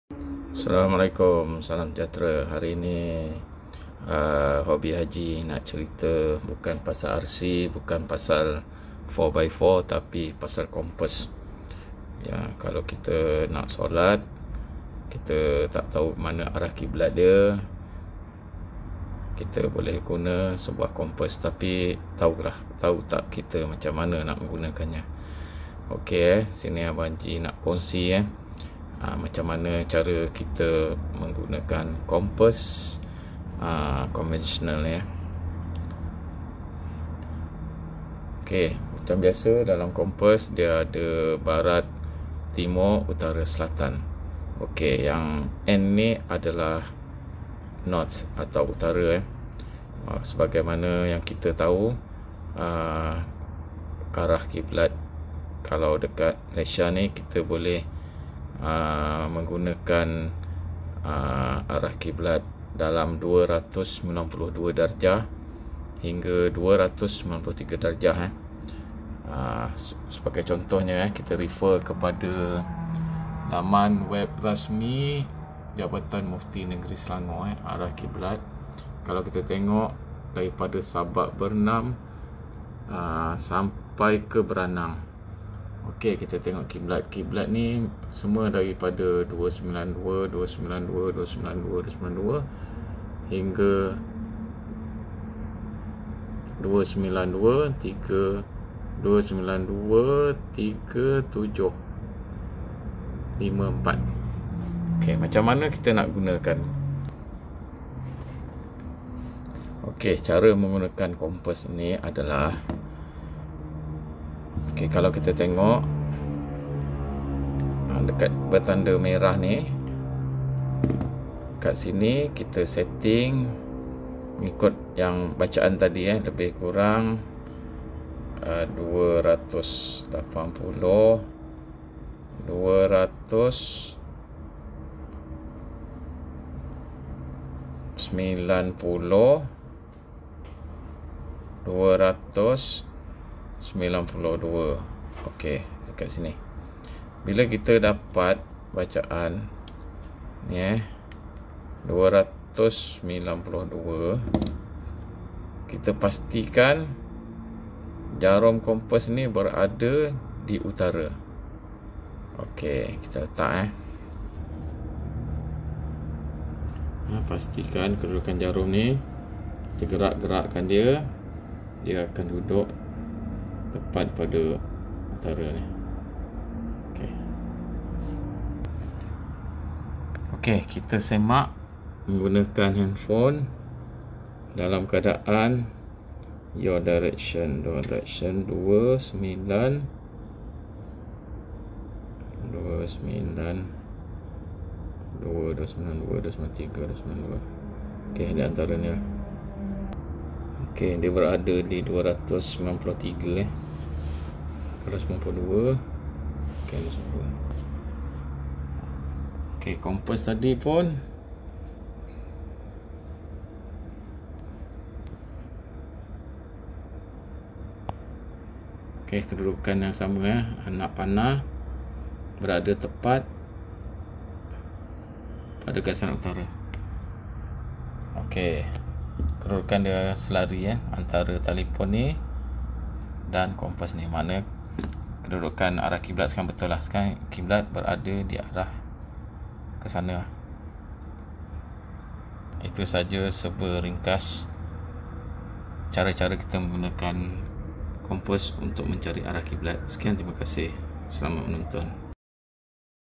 kompaskiblat | Cara guna kompas untuk mencari arah Kiblat
Cara guna kompas biasa utk menentukan kiblat